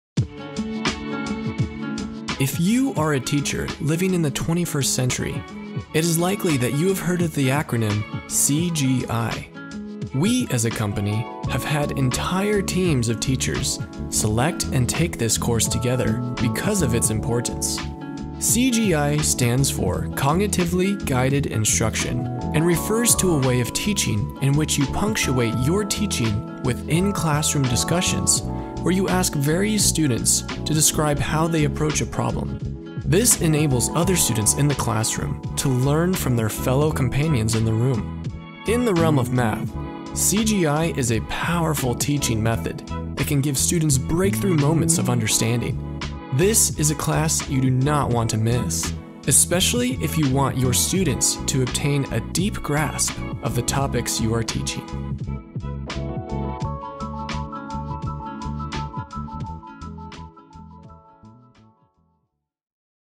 CGI Math: Cognitively Guided Math Instruction | Course Sample
Enjoy a sample of our online professional development course entitled: CGI Math: Cognitively Guided Math Instruction